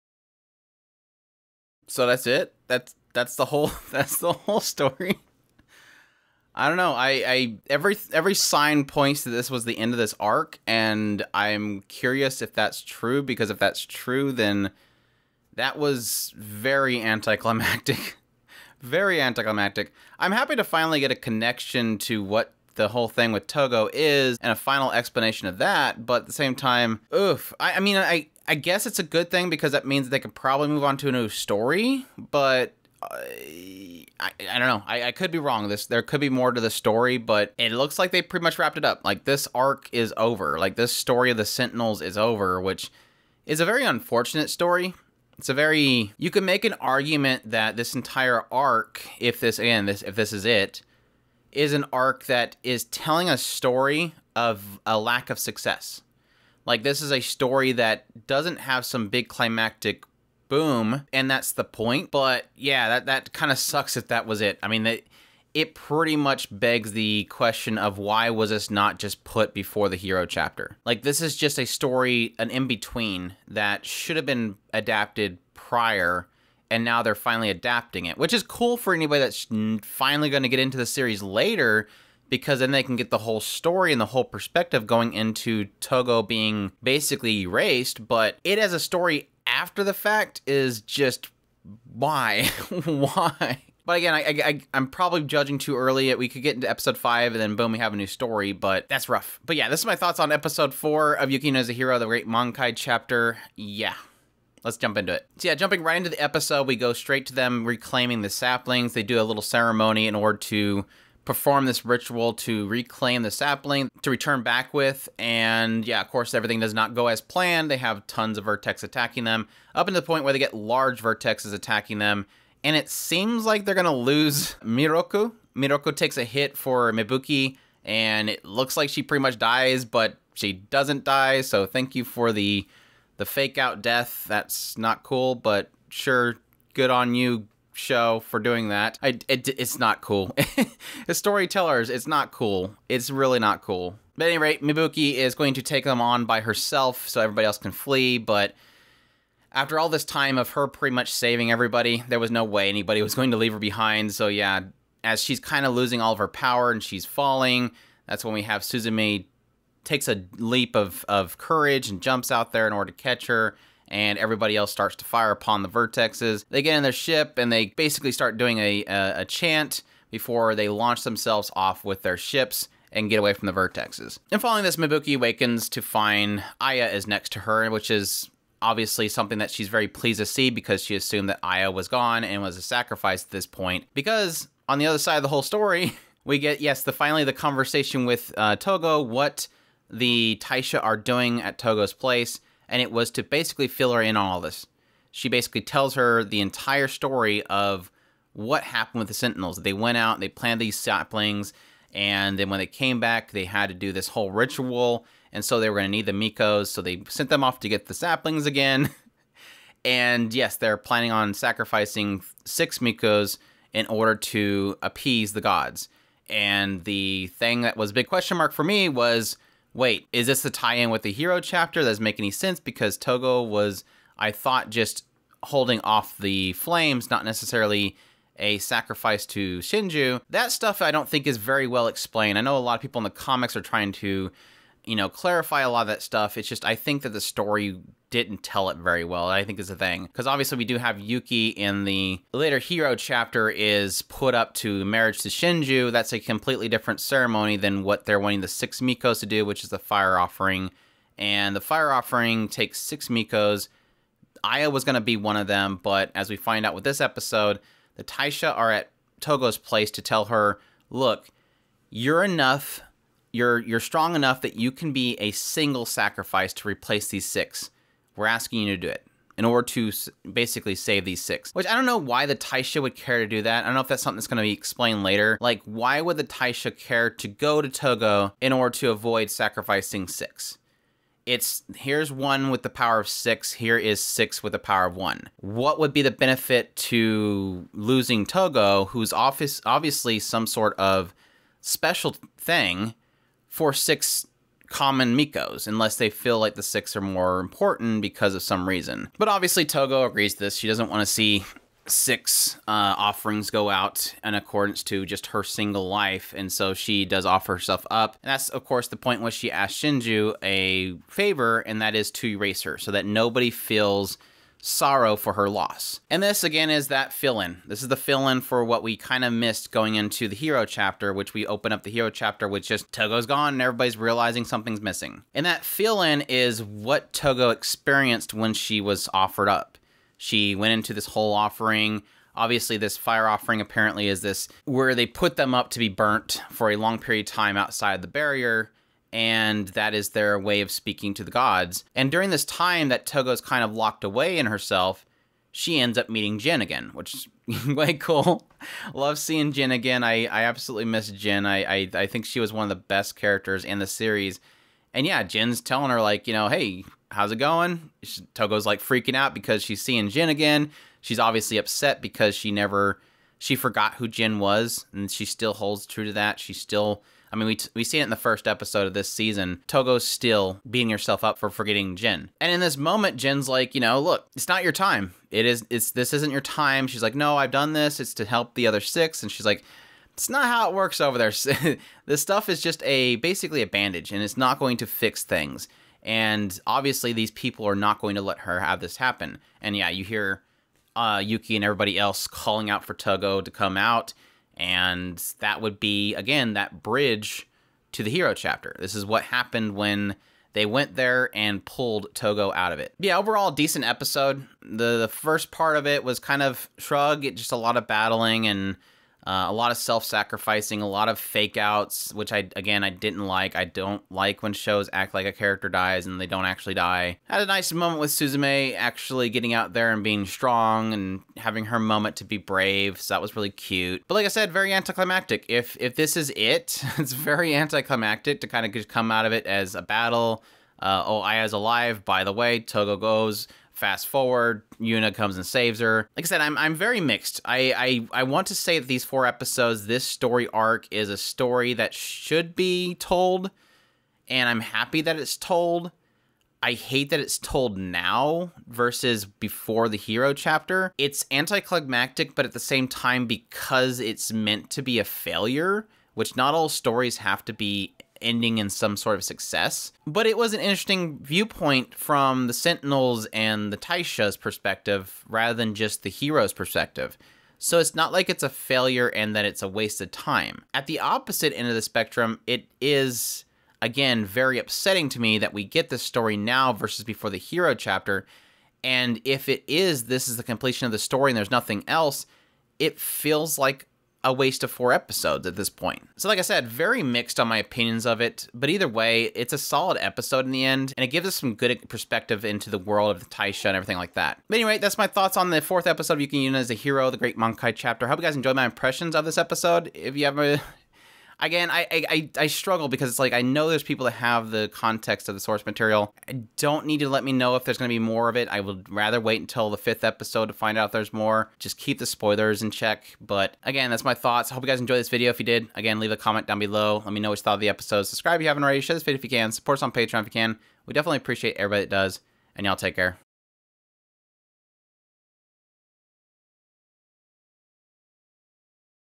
Episode 4 Impressions: Yuki Yuna is a Hero The Great Mankai Chapter (Dai Mankai no Shou)
Episode 4 Impressions of Yuki Yuna is a Hero The Great Mankai Chapter.  Japanese title Yuuki Yuuna wa Yuusha de Aru: Dai Mankai no Shou.